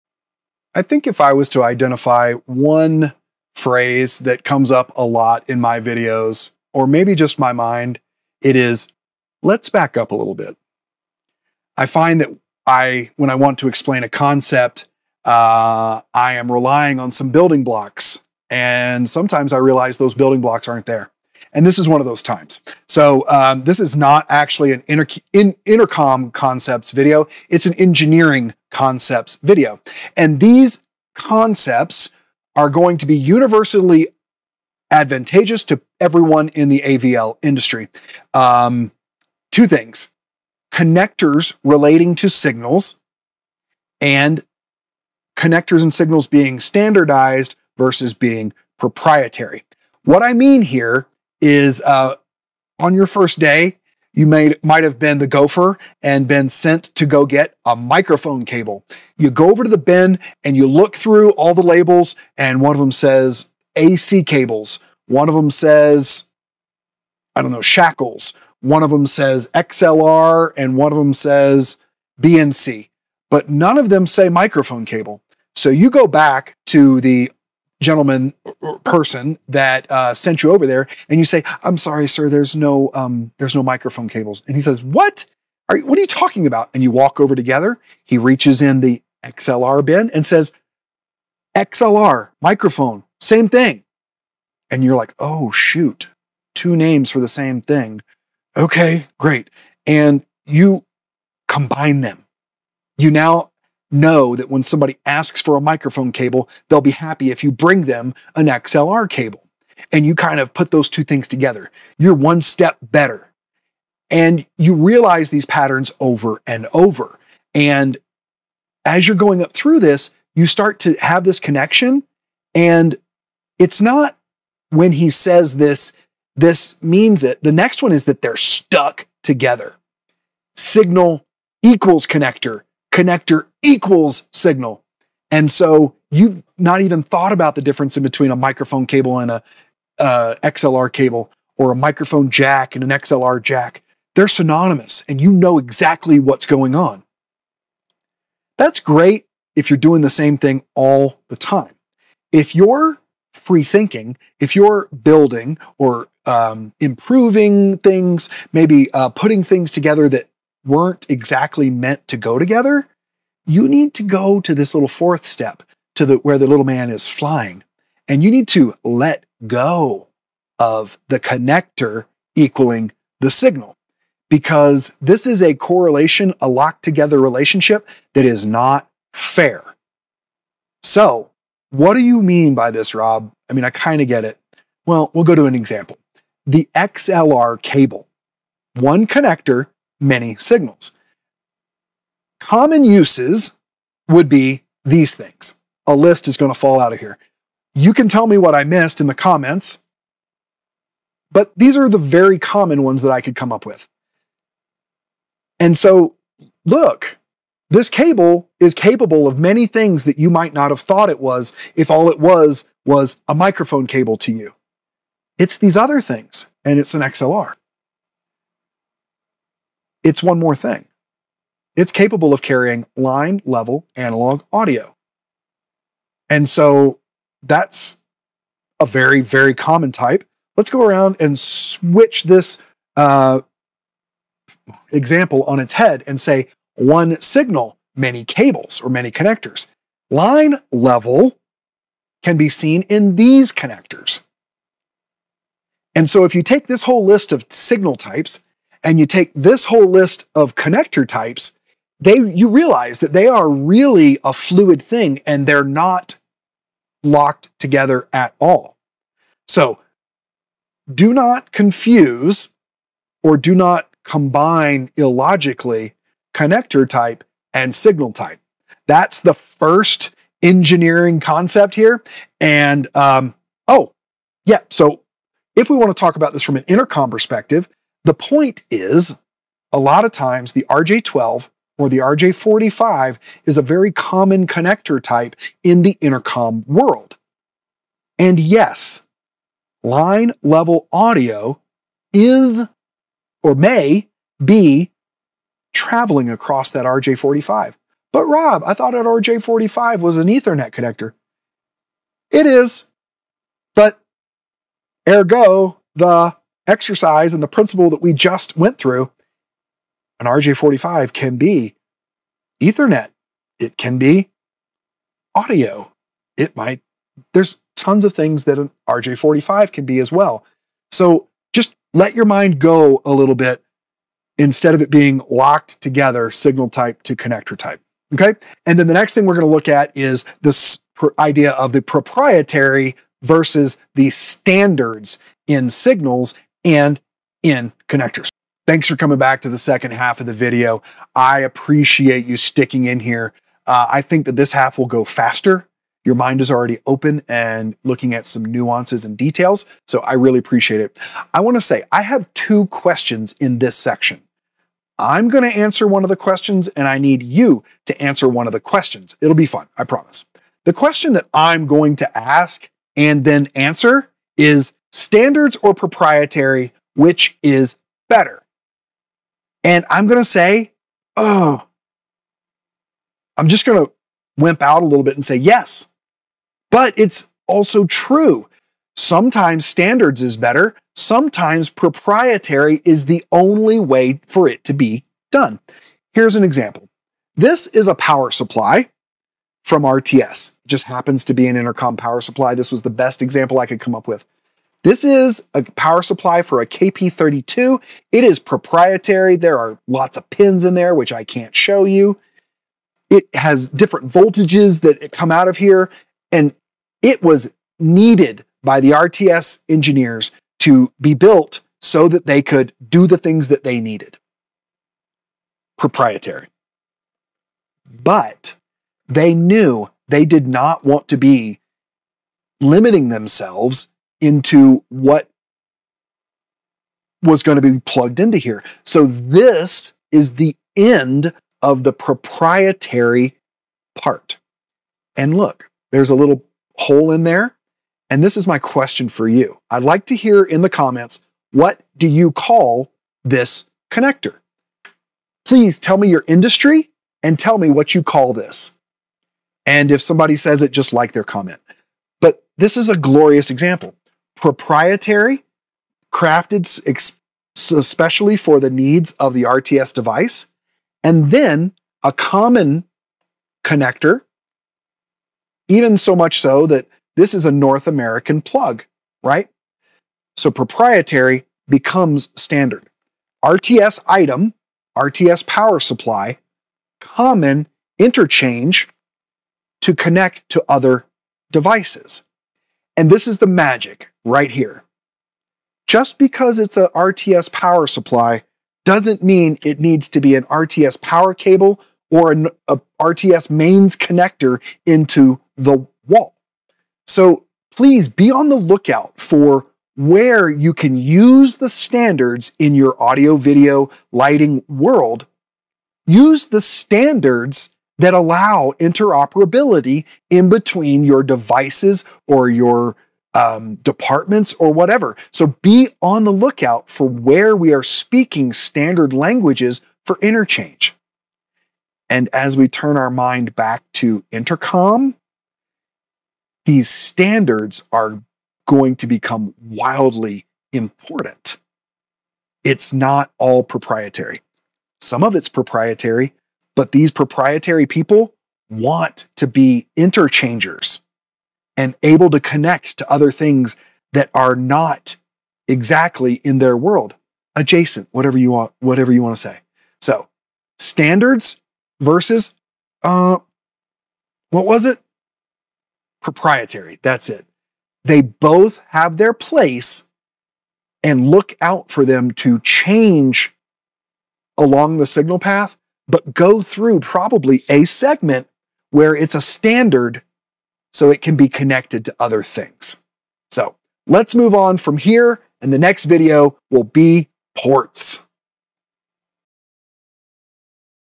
Concepts - ep0.5 Engineering Mindset
Setting our mindset as we think about engineering concepts of standard and proprietary concepts and signals